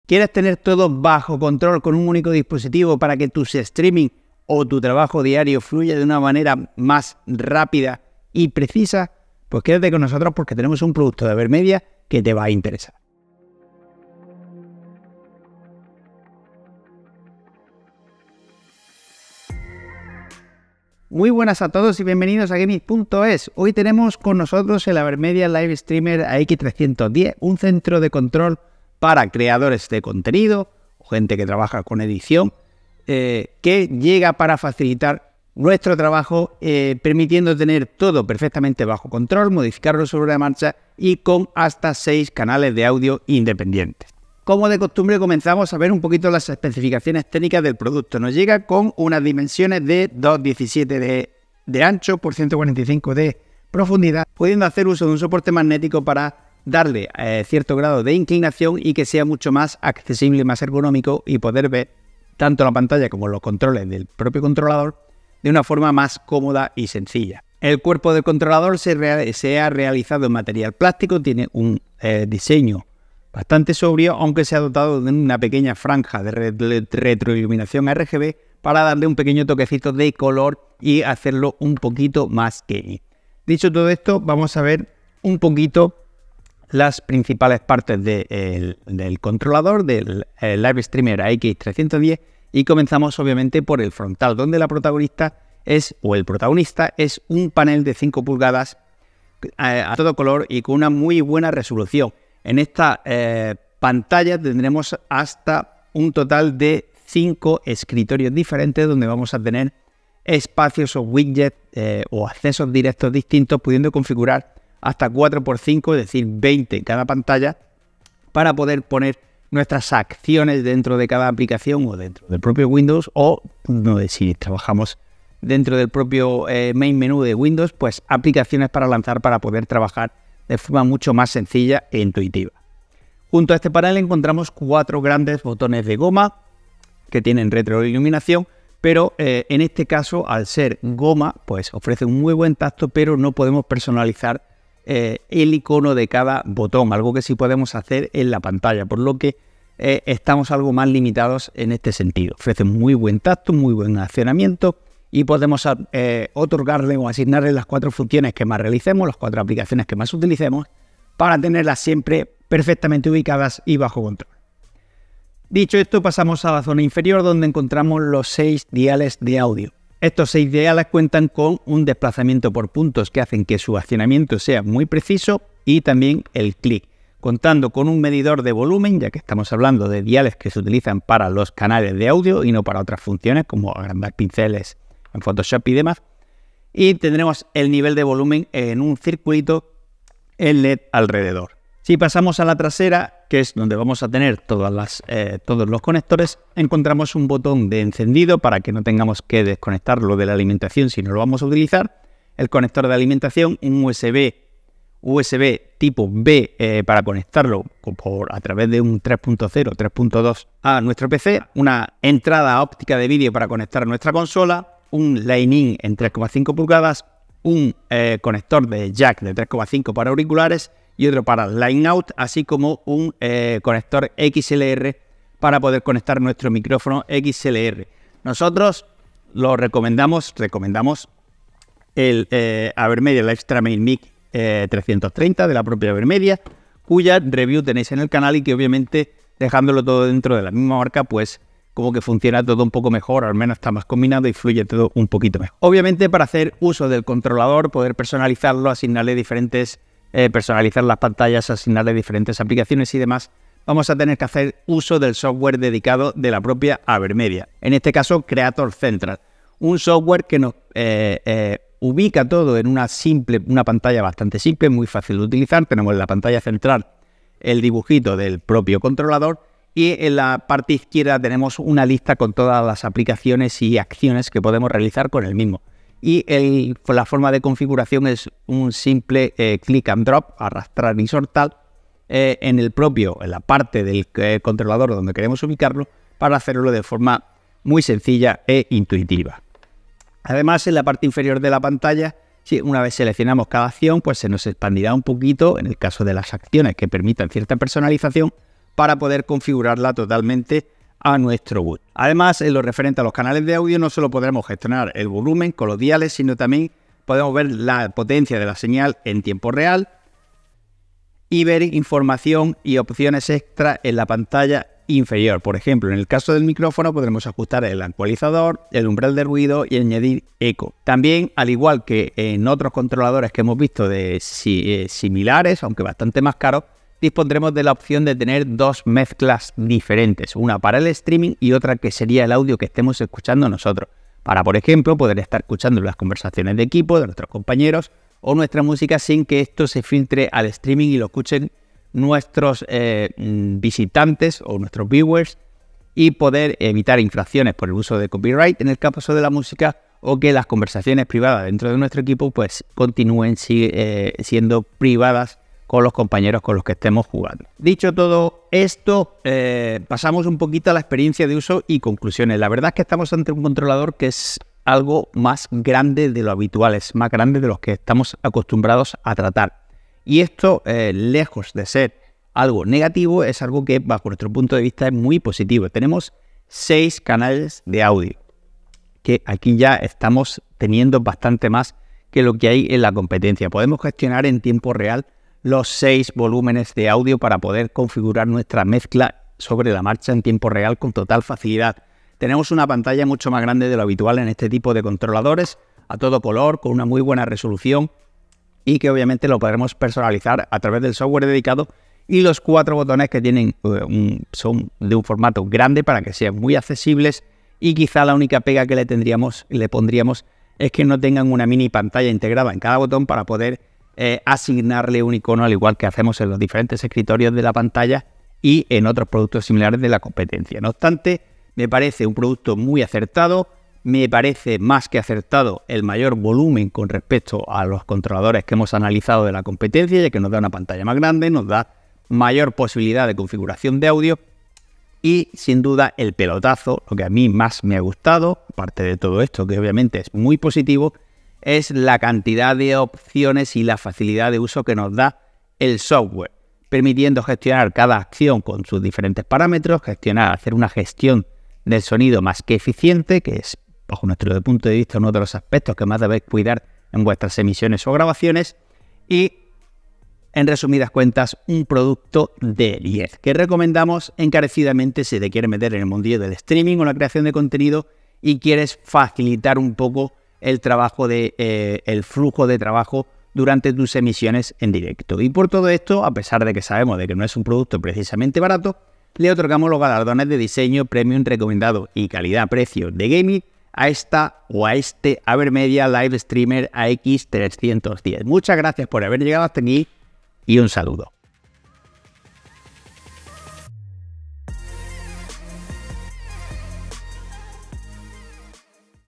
AverMedia Live Streamer AX310, analizamos el controlador de streaming más completo del mercado
Review en GameIt ES en una nueva pestaña)

Analizamos el AverMedia Live Streamer AX310, un centro de control para que crees contenido de forma fluida y premium